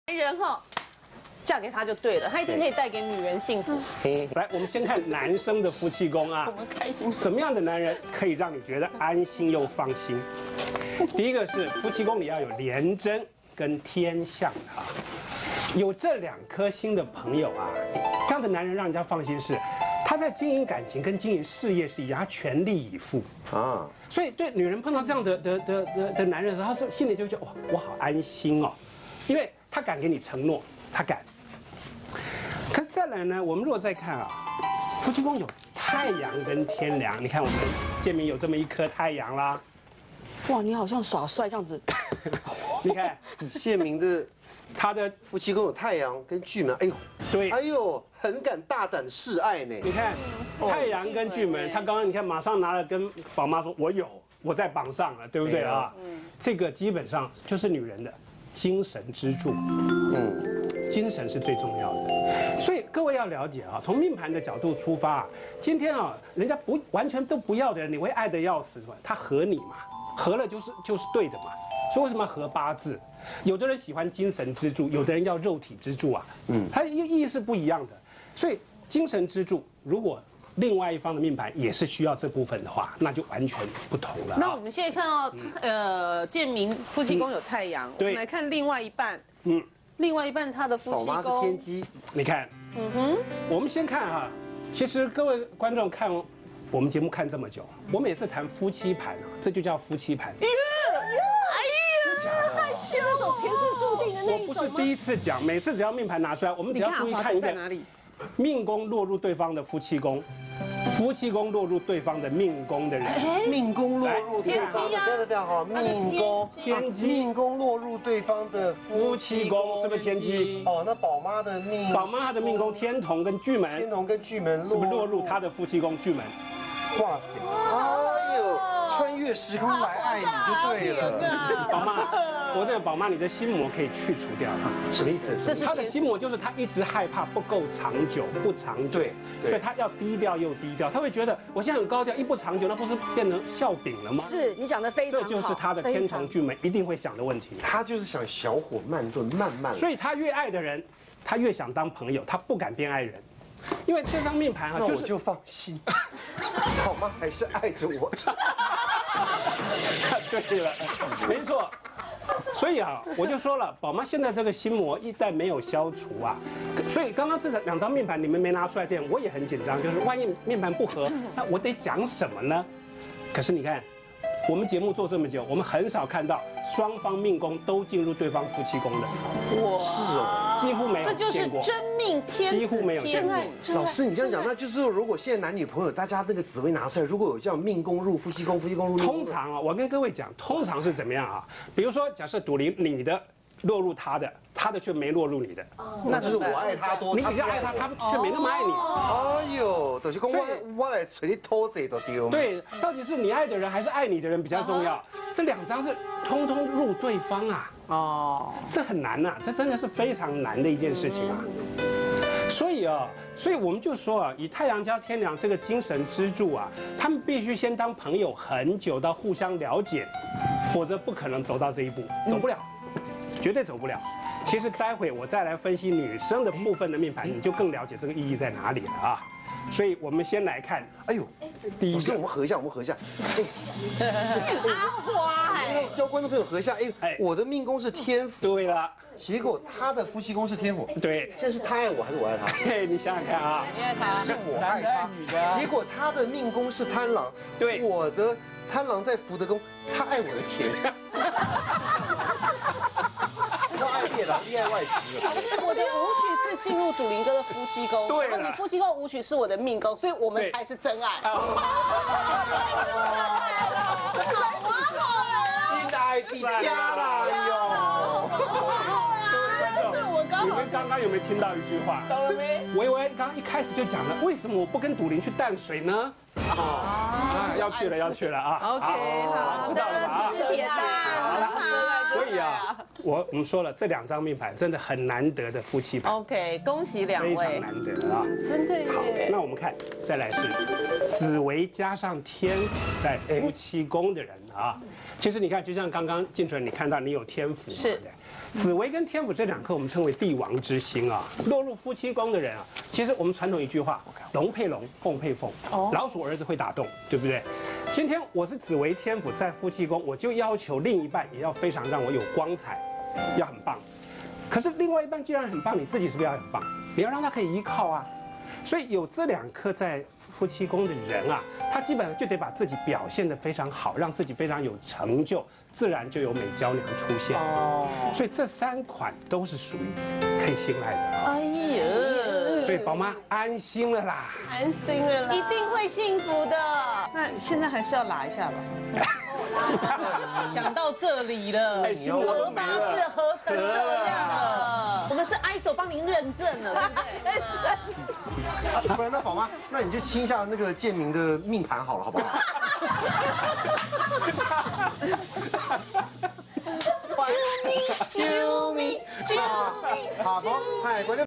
【張盛舒老師】什麼紫微命格的男人，可以帶給女人幸福！
【張盛舒老師】什麼紫微命格的男人，可以帶給女人幸福！-20131023
還有寶媽、汪建民紫微命盤分析唷！
還不知道你的紫微命盤嗎？馬上查